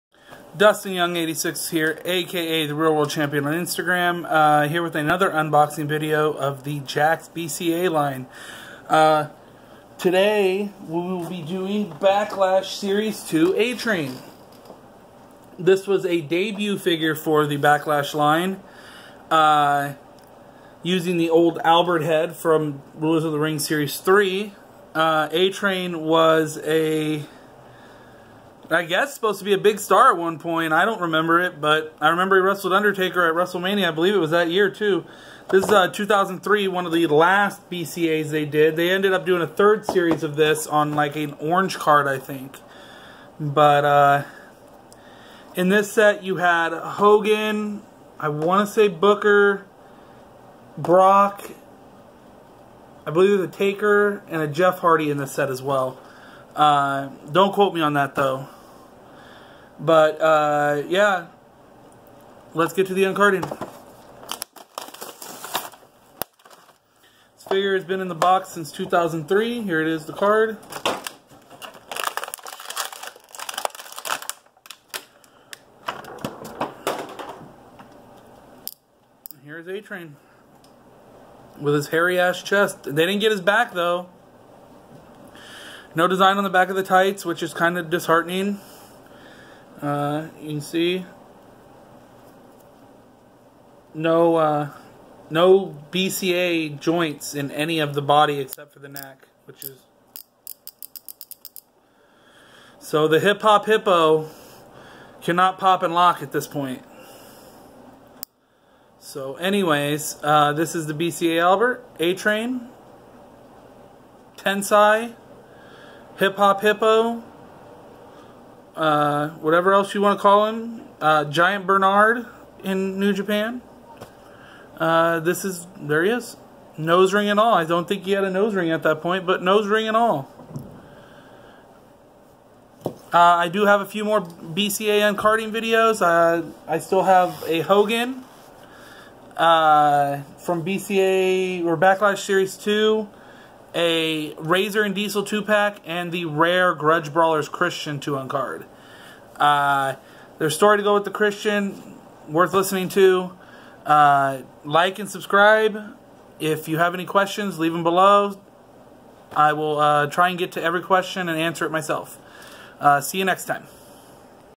WWE WWF Jakks Blacklash Series 2 A-Train (Lord Tensai Albert) BCA Unboxing Uncarding
Video of me Uncarding a Jakks Blacklash Series 2 A-Train.